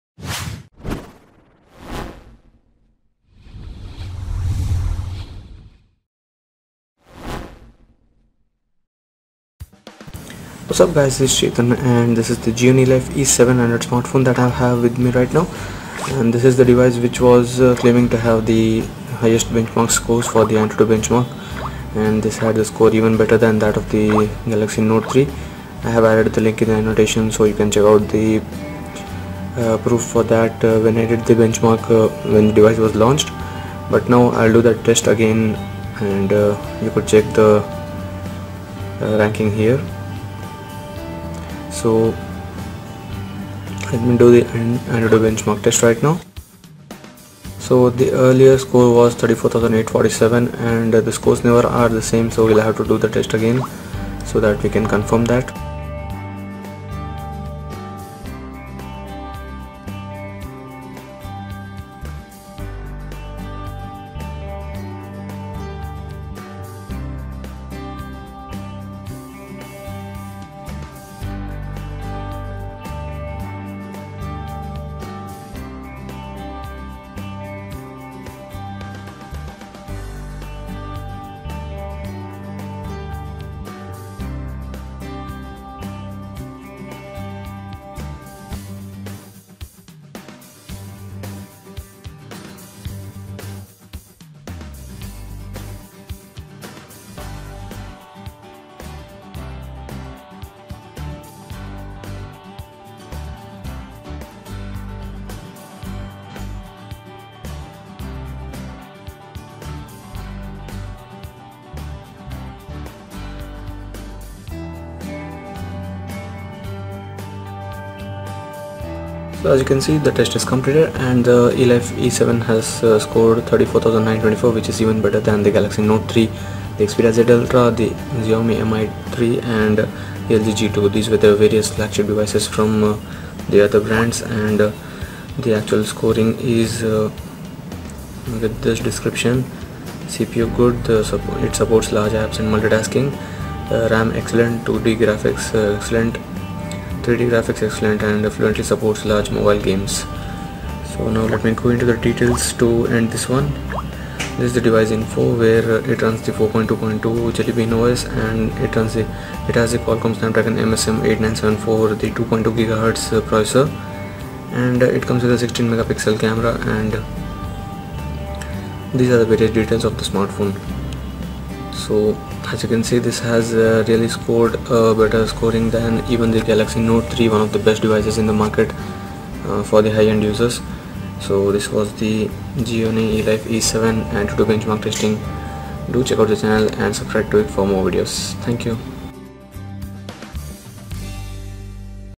Gionee Elife E7 Antutu Benchmark Score - Best Score in List
Gionee Elife E7 got a score of nearly 35000 in the AnTuTu benchmark test that we did. It's one of the best scores that an Android phone has got.